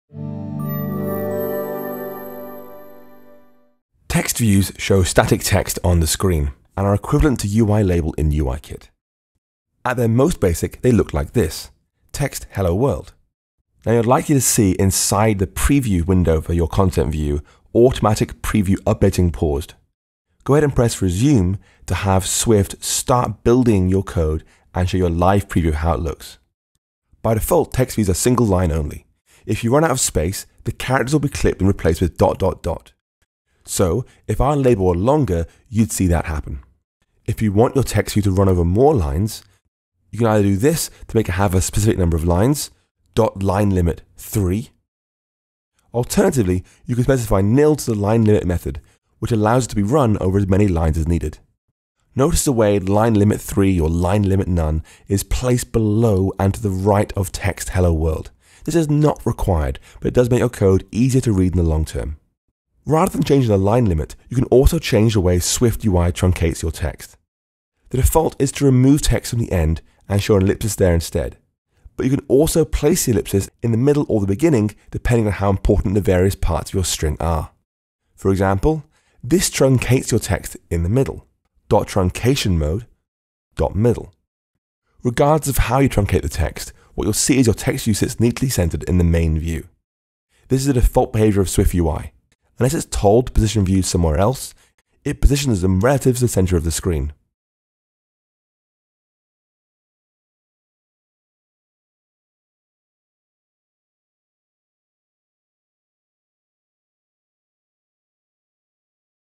How to create static labels with a Text view - SwiftUI by Example
Text views show static text on the screen, and are equivalent to UILabel in UIKit.